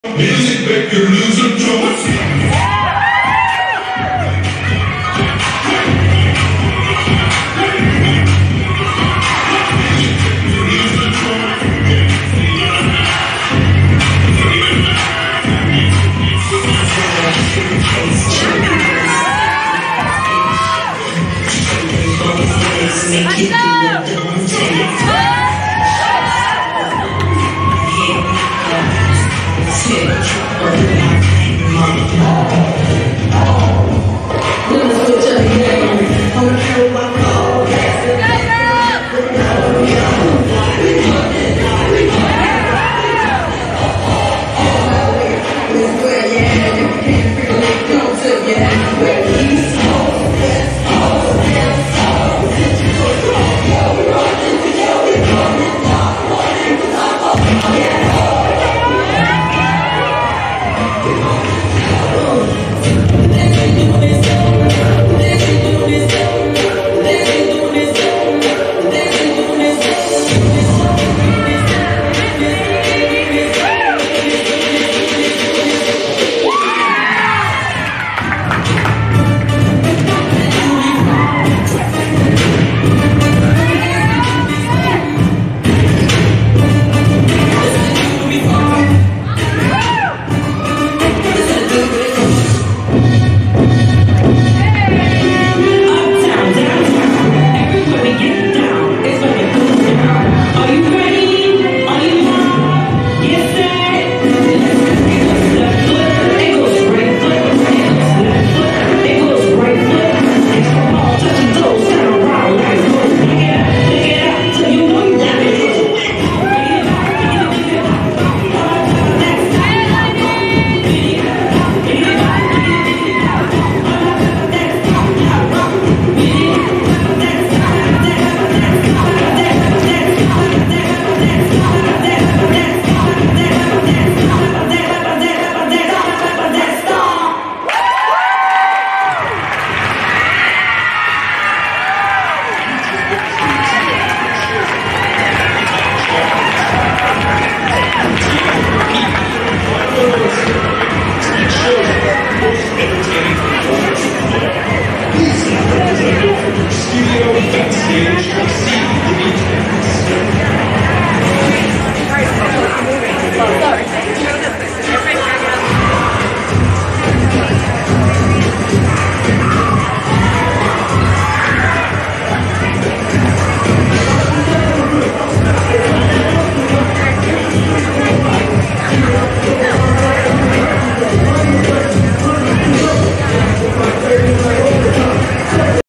🚨PAC ATTACK WINNER🚨 Kansas City 2021 12&Under: PEP Rally - The Studio Liberty Dance and Glee
This routine won the PAC Attack Challenge™️ at our 2021 Kansas City event! 👏Congratulations👏